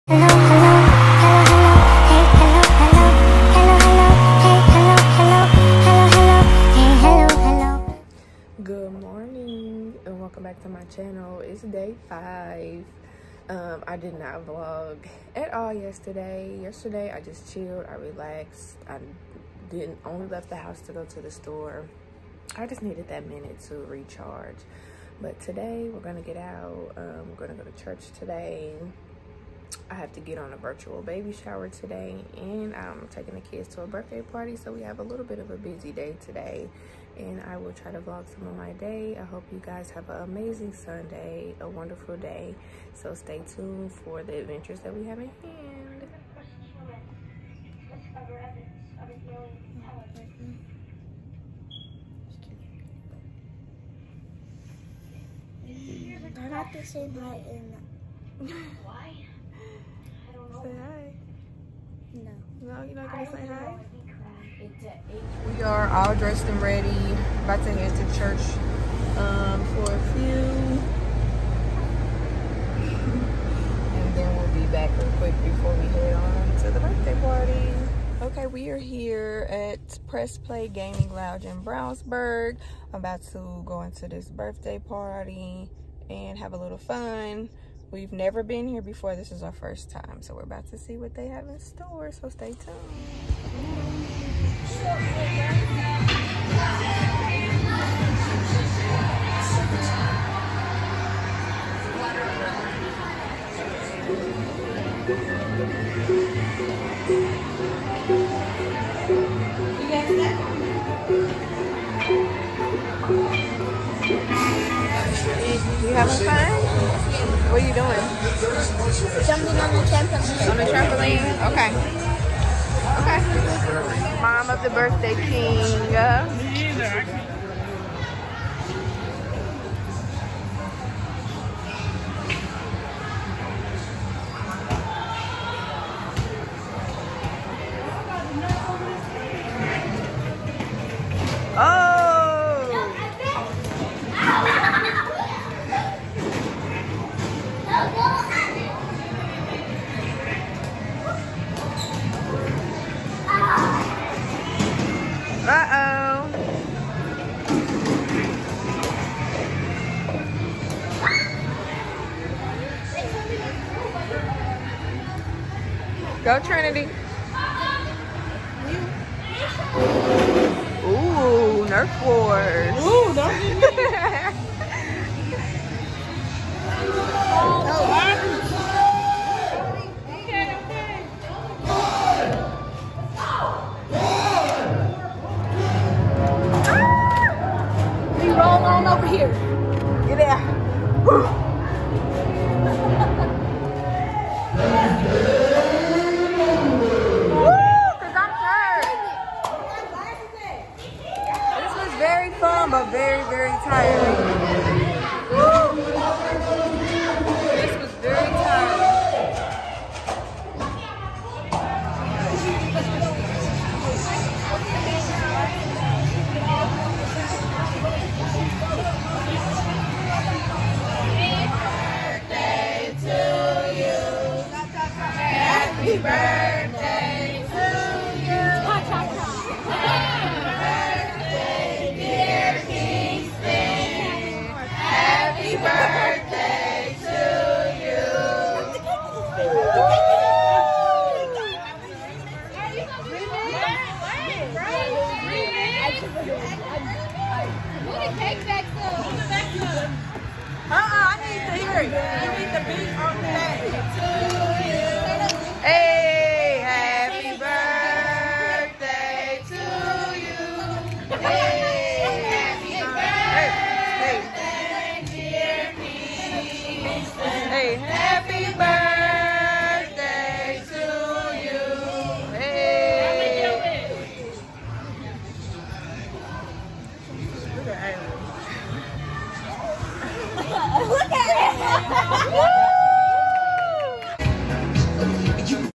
Vlogmas Day 5 Press Play Gaming Lounge
Hey guys come with us as we explore Press Play gaming Lounge for the first time